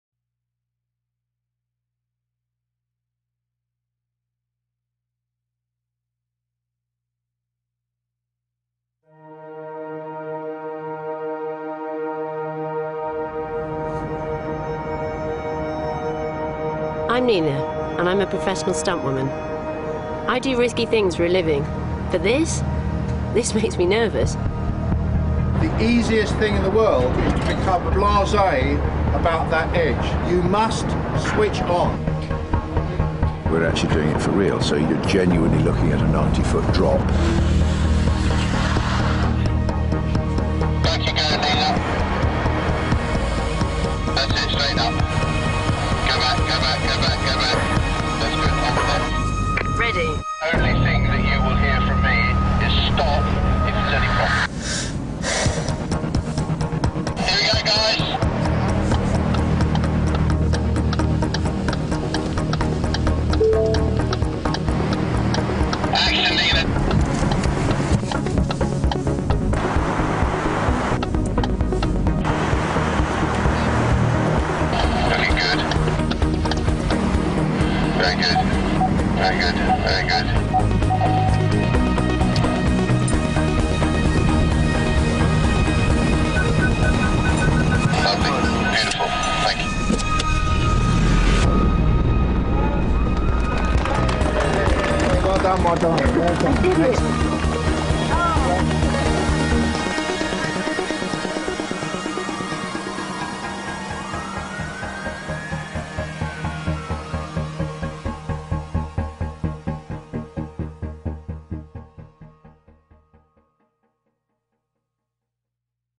Ford Focus Park Assist - Real Life Demo
This video if from Ford Europe, shows a real life customer parking her Focus with Park Assist... on top of a building!

No room for error here!